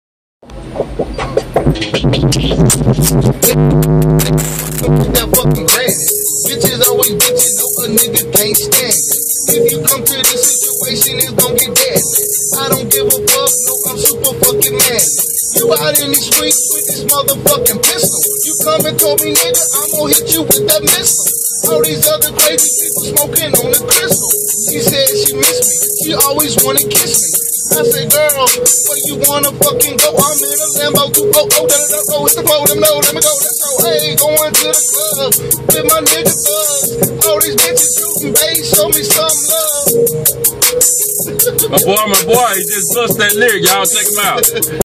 Big Kountry bussing some freestyle lyrics
My home boi is hard ass fuck with this shit